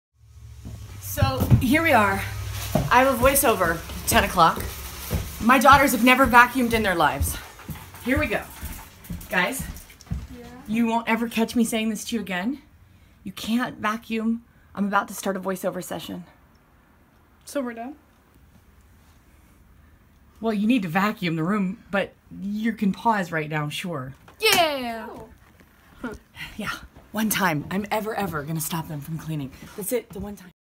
VO Vacuuming
VO Problems - when working at home and the kids are around! Who ever tells teenagers to NOT vacuum their rooms?